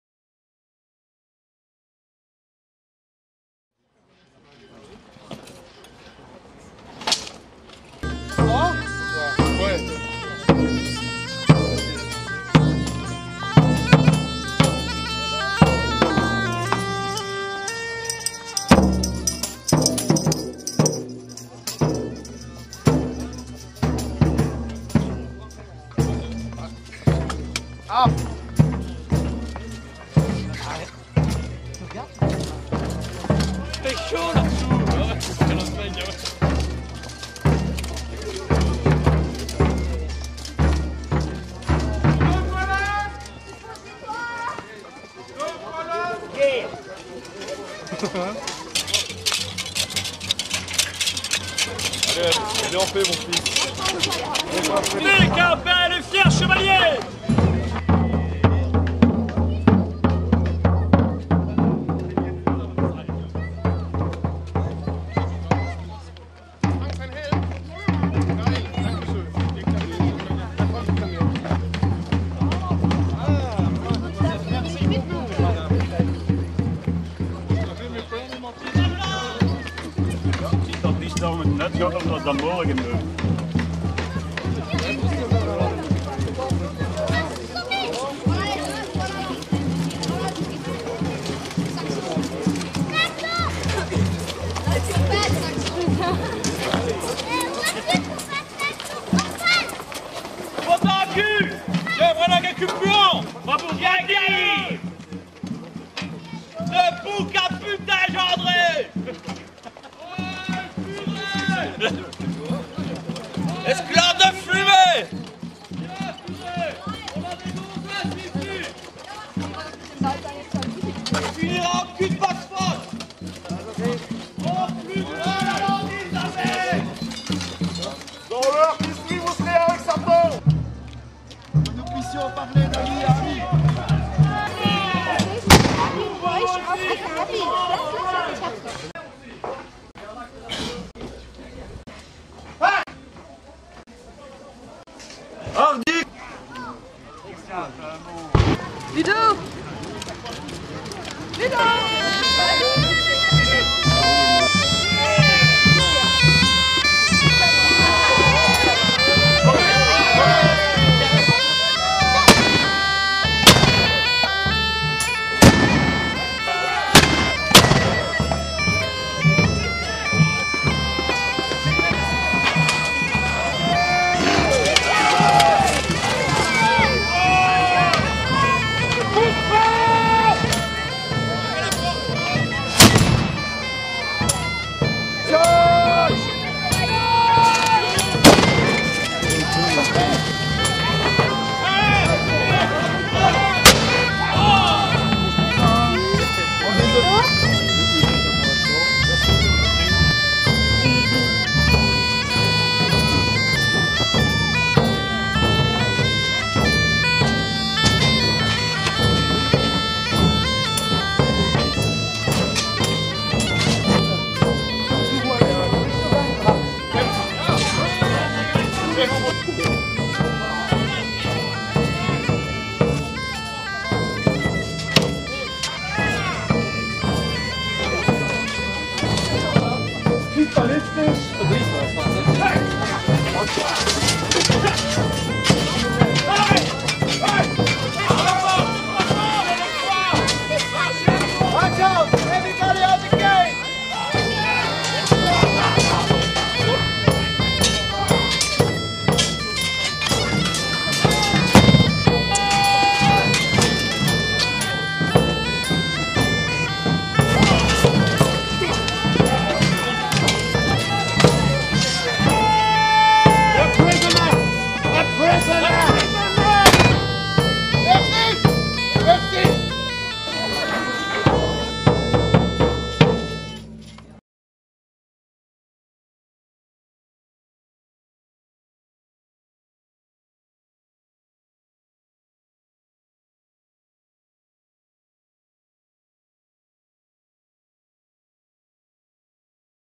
Rassemblement Verte Tente 2003 - attaque de Franchimont
Le 1er mai 2003, nos amis de De Legend (Anvers) nous ont fait la surprise de filmer l'attaque du château de Franchimont à l'occasion du rassemblement de la Verte Tente.